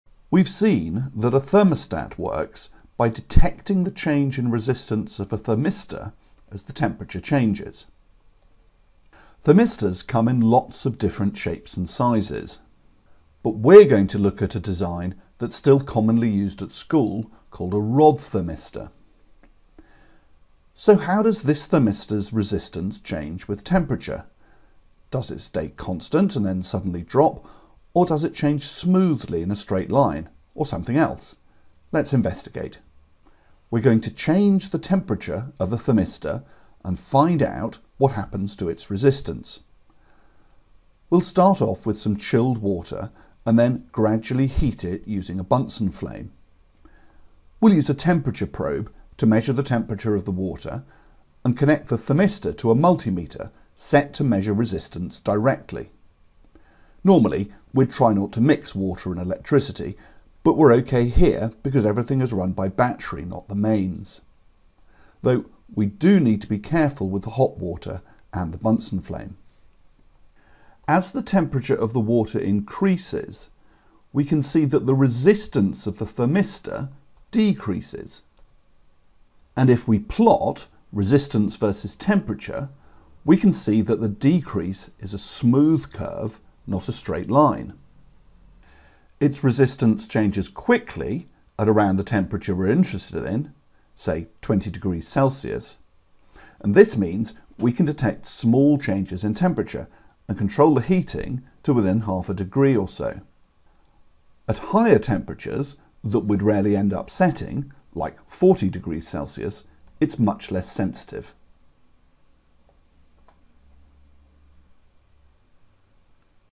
12.4 Thermistor resistance vs temperature investigation
An investigation to show how the resistance of a  thermistor changes with temperature